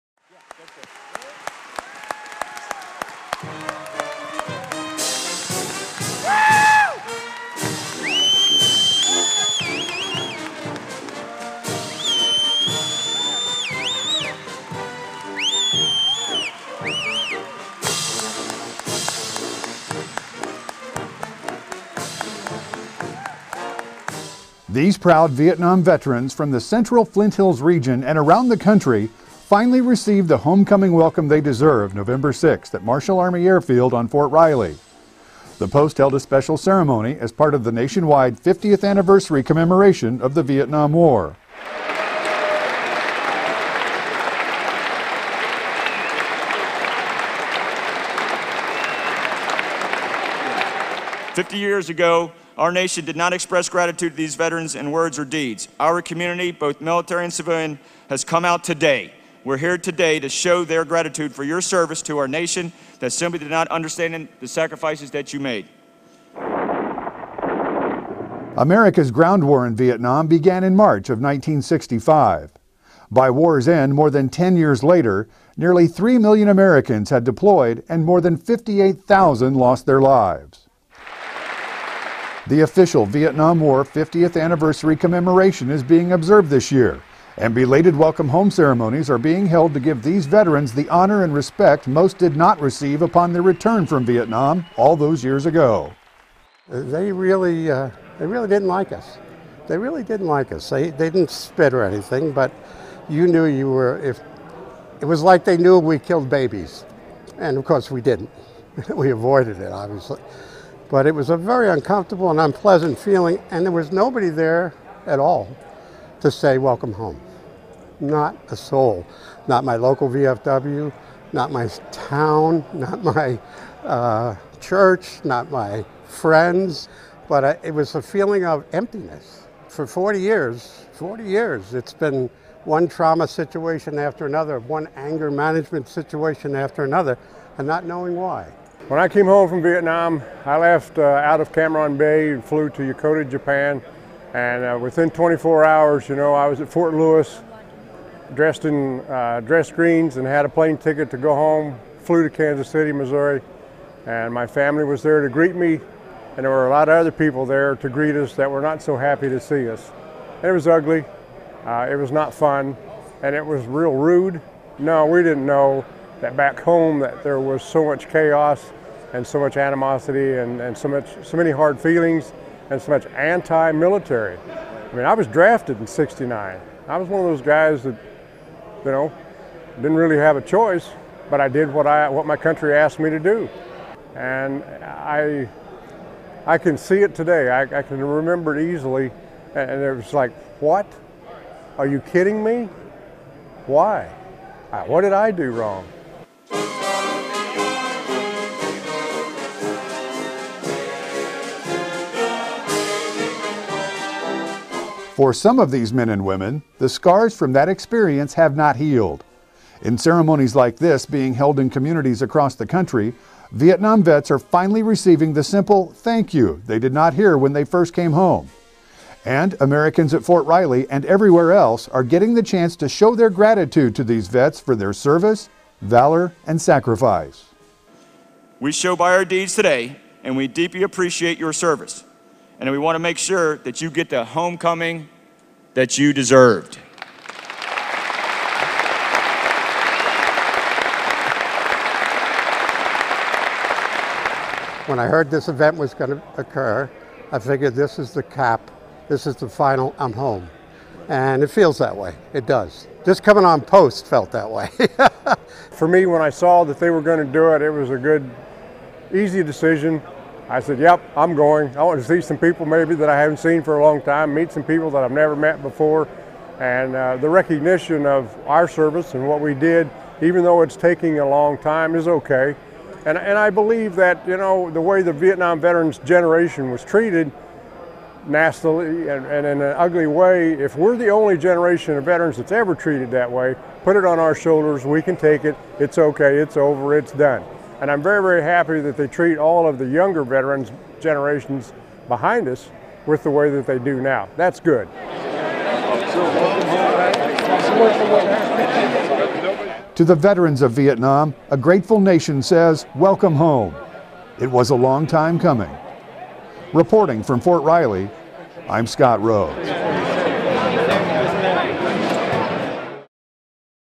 Vietnam War Veterans Welcomed Home
U.S. Army Soldiers and Fort Riley community members honor Vietnam veterans during a welcome home ceremony hosted by 1st Infantry Division at Fort Riley’s Marshall Army Airfield, Kan., Nov. 6, 2015.  "It's all about finally coming home… this is the final I'm home,” said John Moen, a combat medic assigned to 3rd Brigade Combat Team, 1st Infantry Division from 1966 to 1967.  The ceremony, held in commemoration of the 50th Anniversary of the ‪‎Vietnam War‬, honored the‬ service, valor and sacrifice of the veterans and formally welcomed them home.

U.S. Army video by Scott Rhodes